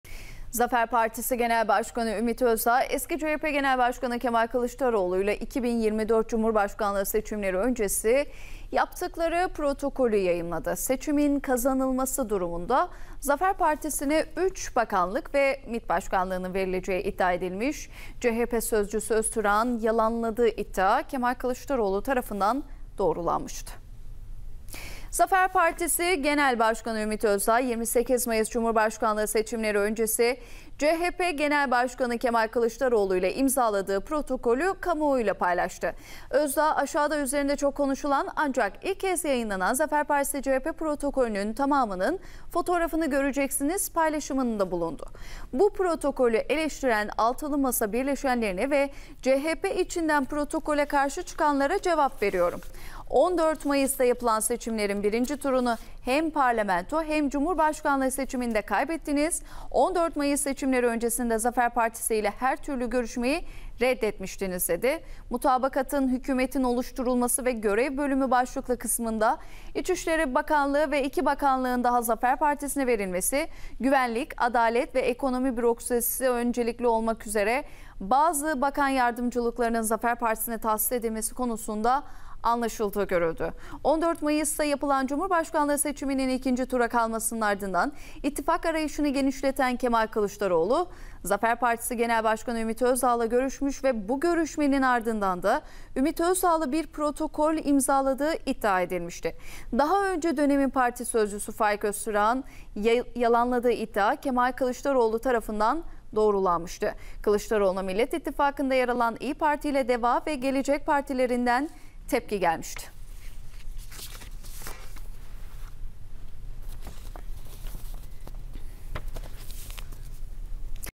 Kemal Kılıçdaroğlu ve Ümit Özdağ Arasındaki Seçim Pazarlığı İfşa Oldu
Ümit Özdağ Kemal Kılıçdaroğlu ile yaptığı gizli protokolün belgelerini paylaştı